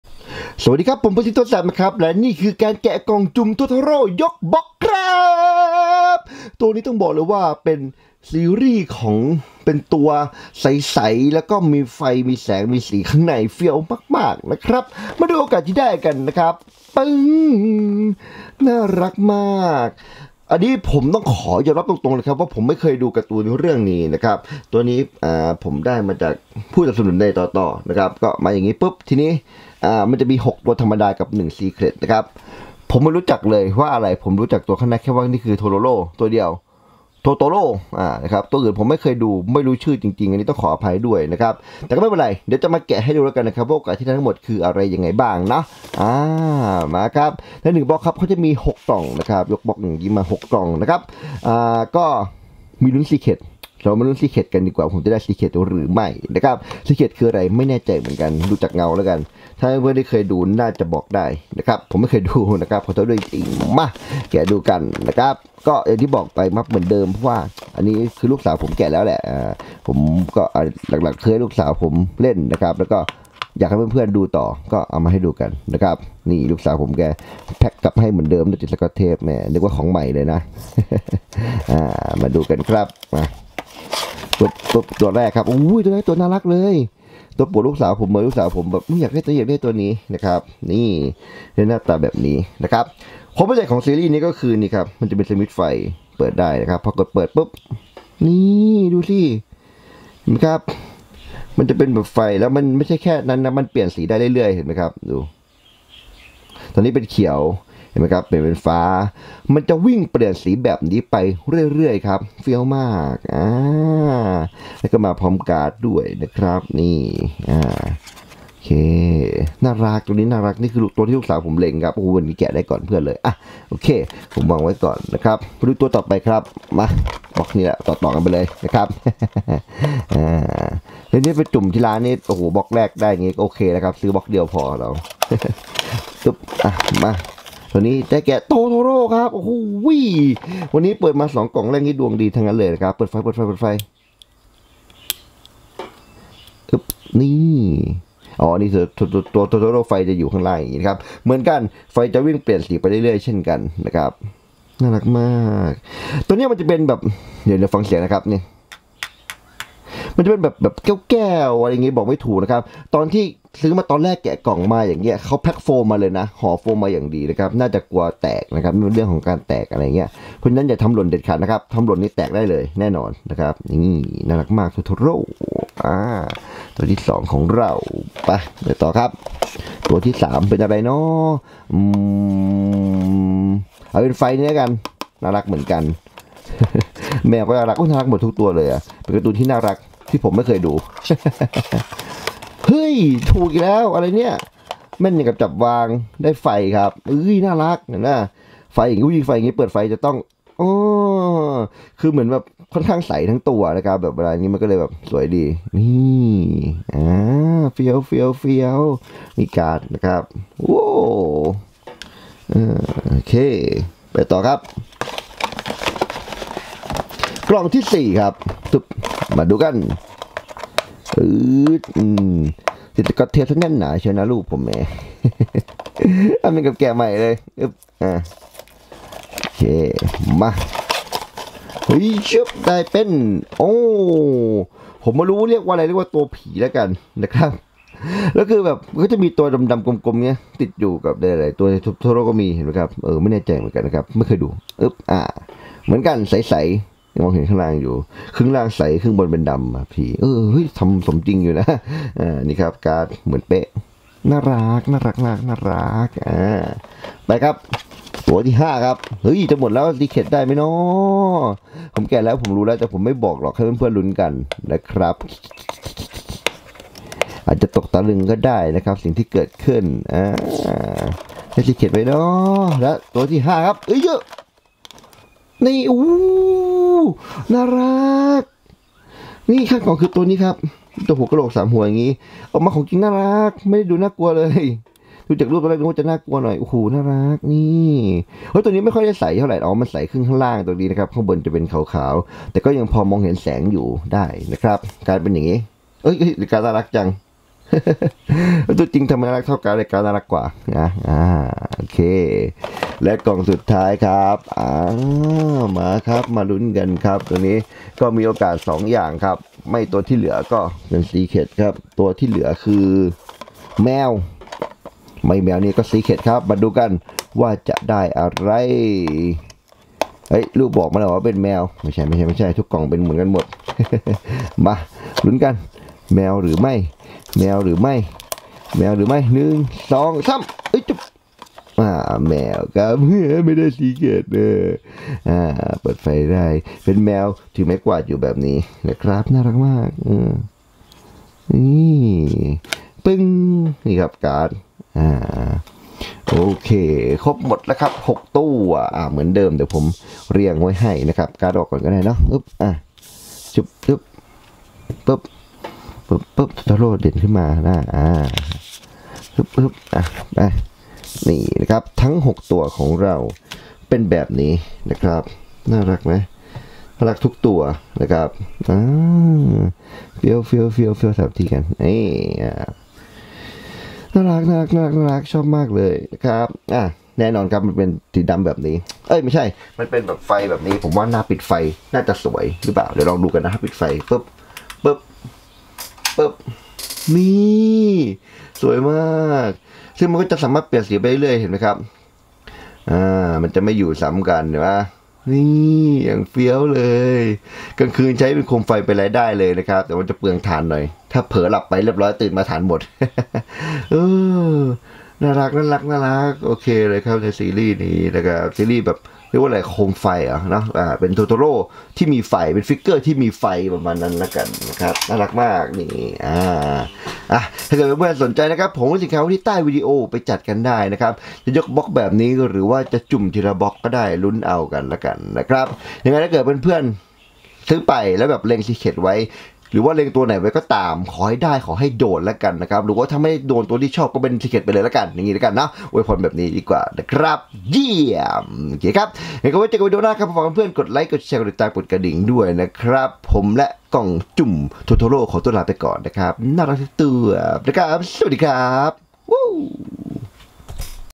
แกะกล่องสุ่ม TOTORO ยกบ็อกซ์ เปิดไฟได้ เปลี่ยนสีเอง | รีวิวของเล่น | เพื่อนซี้ ตัวแสบ 👓
มี 6 ตัวธรรมดาและ 1 Secret ครับ สำหรับซีรี่ย์นี้ มันจะเป็นตัวโมเดลใสๆเปิดแสงไฟได้ และแสงไฟจะเปลี่ยนสีวนๆไปเรื่อยๆ เล่นตอนกลางคืนแล้วสวยมากครับ